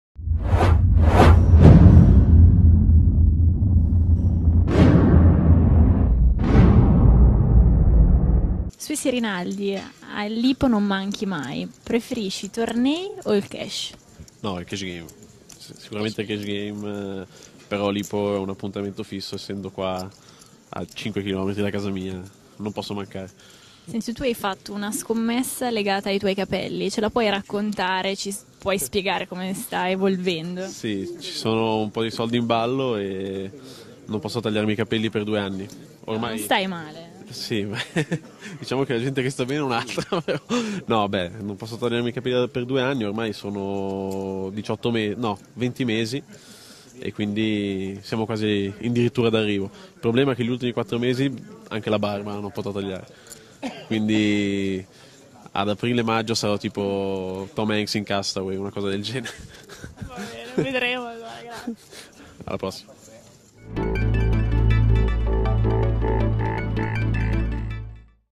IPO Campione 13, intervista a Claudio "swissy" Rinaldi
Claudio "swissy" Rinaldi è un giocatore fisso dell'Italian Poker Open, e infatti lo ritriviamo anche qui all'IPO 13.

Nonostante preferisca il poker in modalità cash game, Rinaldi non rinuncia all'IPO, uno dei suoi tornei live preferiti.

E i capelli? una scommessa che terminerà fra 4 mesi e che gli dovrebbe fruttare soldi...